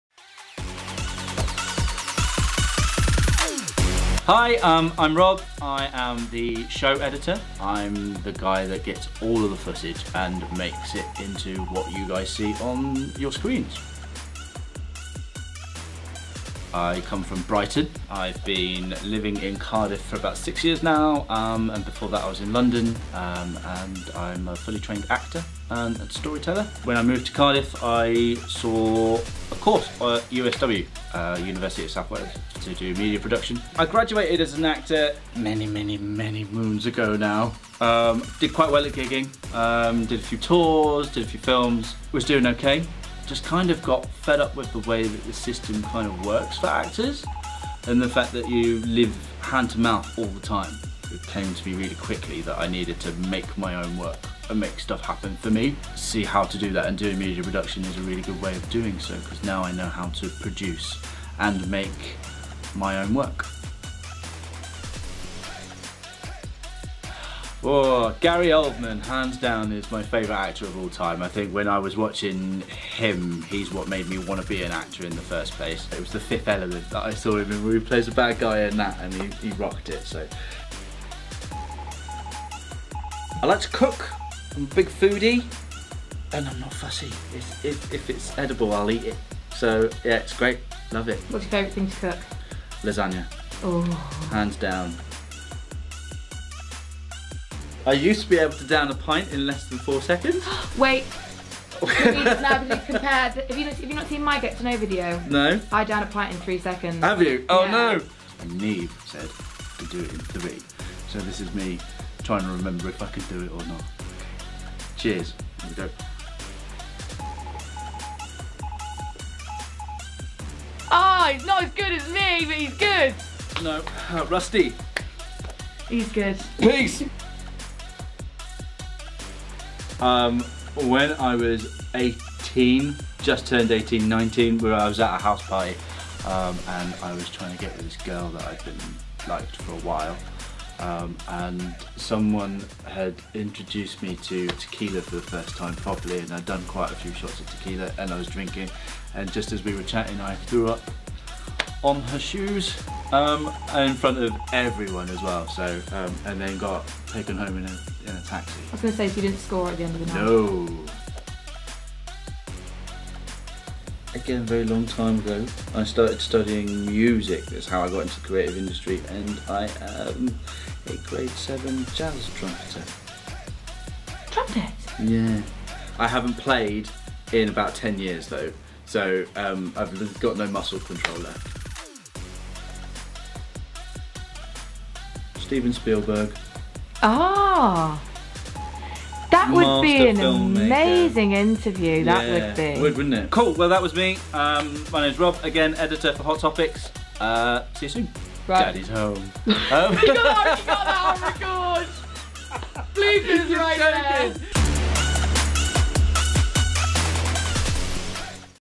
Getting to Know - Robin H Jones, Show Editor
Starting his career as an actor, Robin has now retrained in production and has a strong ambition to become a director, potentially even the next Steven Spielberg or Christopher Nolan.

As an actor, the part he enjoys most is being the bad guy. Inspired by the likes of Gary Oldman, Robin says it's more of a challenge and takes a stronger skill set to dive into darker characters.

Away from his career, Robin, is a really cool guy to be around. He's a little naughty in a cool way, and he's a big family man - a terrific father and husband.